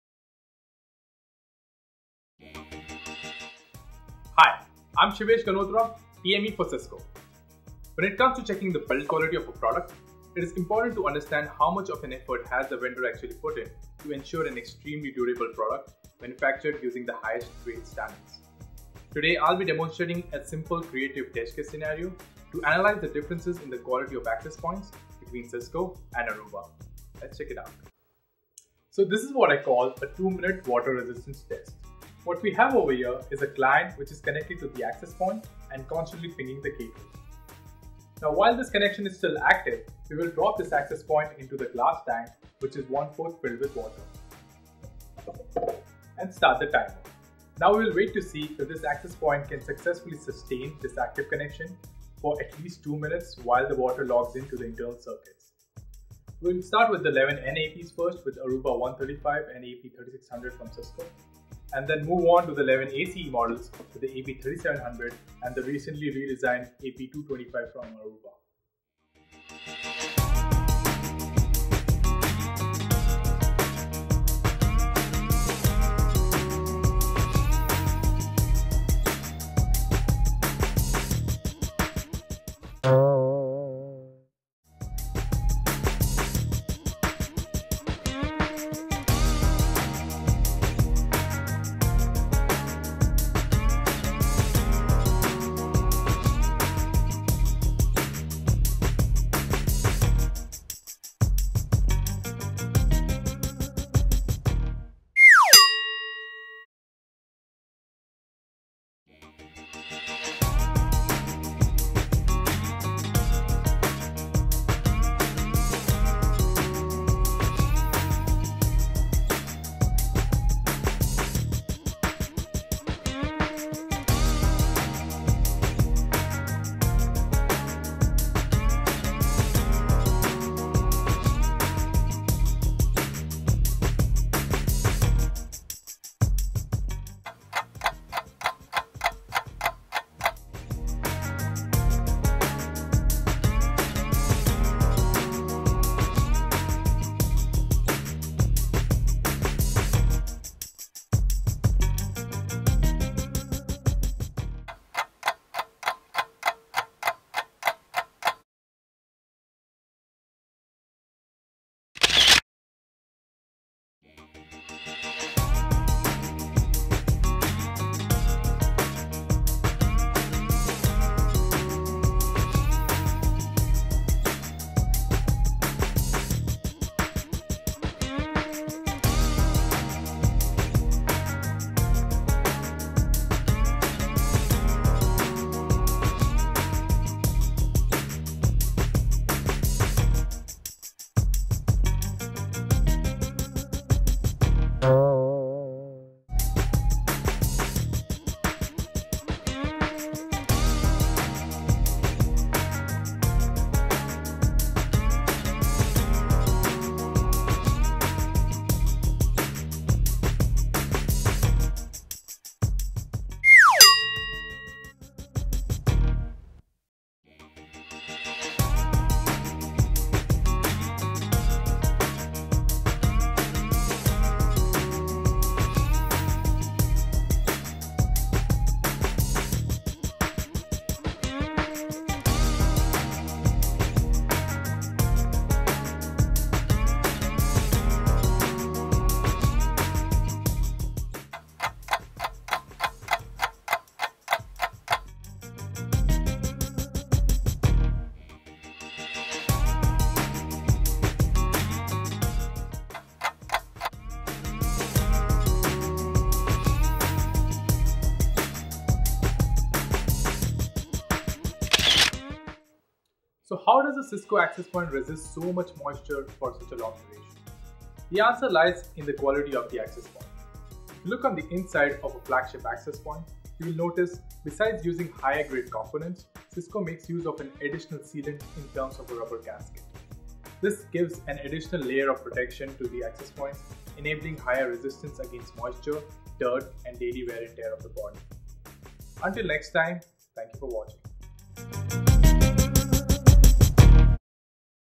Cisco vs. Aruba: AP Quality Matters
By using water as an example, check out how Cisco's Flagship APs successfully achieve the higher durability stamp as compared to the Aruba's Flagship APs. The water resistance test case scenario sets an example which shows the amount of design research and efforts that Cisco puts in to deliver the most resilient, best in class access points in the WLAN market